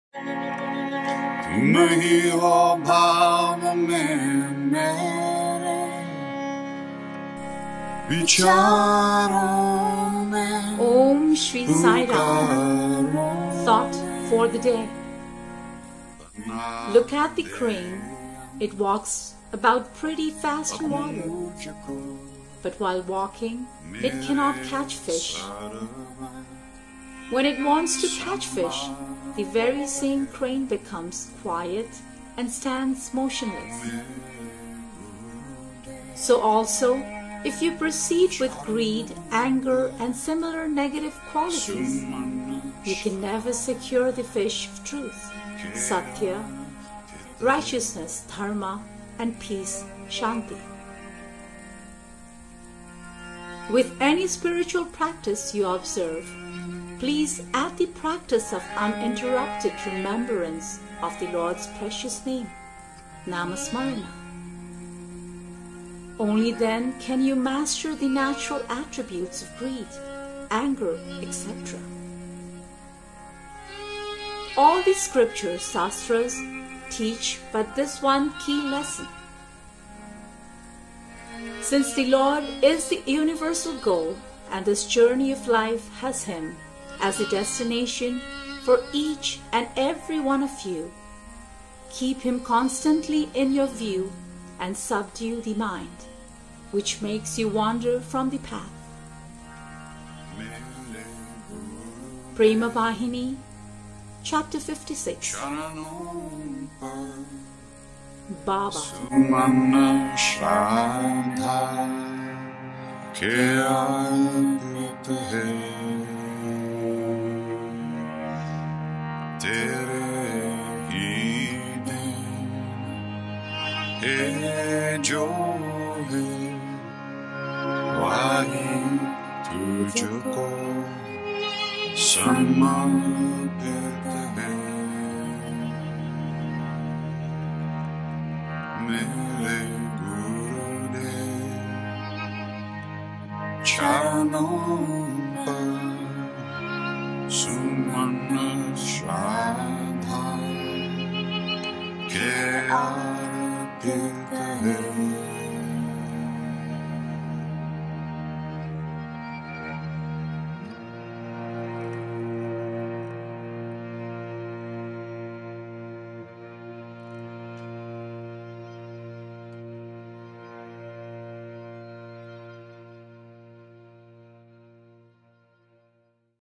Mensaje de la Pizarra de Prashanti Nilayam_23 de junio de 2015.
Activar subtitulos en el reproductor abajo a la derecha, seleccionar idioma español.
Courtesy_Radio Sai and Thought For The Day by Sai Art.

MENSAJE DEL DIA 23 de junio de 2015

Mira a la grulla, que camina bastante rápido en el agua. Pero mientras camina, no puede atrapar peces. Cuando quiere atrapar peces, la mismísima grulla se torna tranquila y permanece inmóvil. Así también, si prosiguen con la codicia, la ira y cualidades negativas similares, nunca podrán asegurarse el pez de la verdad (sathya), la rectitud (dharma) y la paz (shanti). A cualquier práctica espiritual que observen, añadan la práctica del recuerdo ininterrumpido del precioso nombre del Señor (namasmarana). Sólo entonces podrán ustedes dominar los atributos naturales de la codicia, la ira, etc. ¡Todas las escrituras (sastras) no enseñan sino esta lección clave! Puesto que el Señor es el objetivo universal y este viaje de la vida le tiene a Él como destino para todos y cada uno de ustedes, manténganle a Él constantemente en su punto de mira y dominen su mente, que les hace desviarse del camino. (Prema Vahini, Capítulo 56).

Look at the crane; it walks about pretty fast in water. But while walking, it cannot catch fish. When it wants to catch fish, the very same crane becomes quiet and stands motionless. So also, if you proceed with greed, anger, and similar negative qualities, you can never secure the fish of truth (sathya), righteousness (dharma), and peace (shanti). With any spiritual practice you observe, please add the practice of uninterrupted remembrance of the Lord’s precious name (namasmarana). Only then can you master the natural attributes of greed, anger, etc. All the scriptures (sastras) teach but this one key lesson! Since the Lord is the universal goal and this journey of life has Him as the destination for each and every one of you, keep Him constantly in your view and subdue the mind, which makes you wander from the path. (Prema Vahini Ch 56)

— BABA